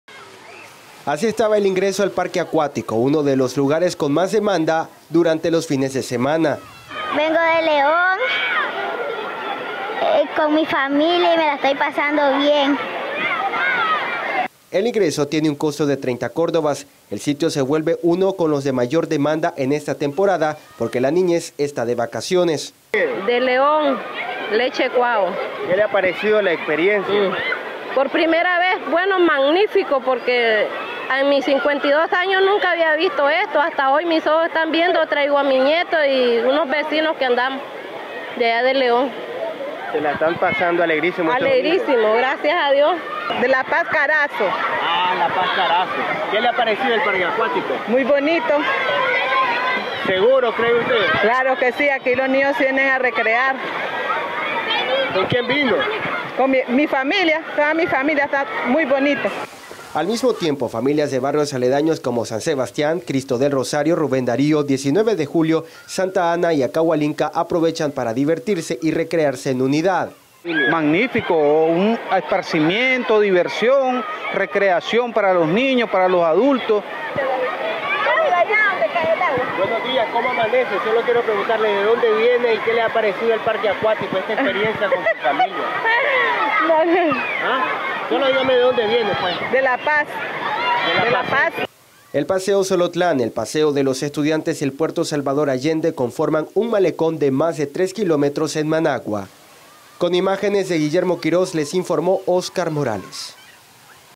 Familias de Oriente y Occidente de Nicaragua visitan Parque Acuático
Este domingo se disfrutó en familia en uno de los espacios más gratificantes, el parque Acuático de Managua, prueba de ellos, son las familias enteras que han llegado desde oriente y occidente en buses para pasar la mañana en el Paseo Xolotlán, espacio que integra el malecón de más de 3 kilómetros en la ciudad capital.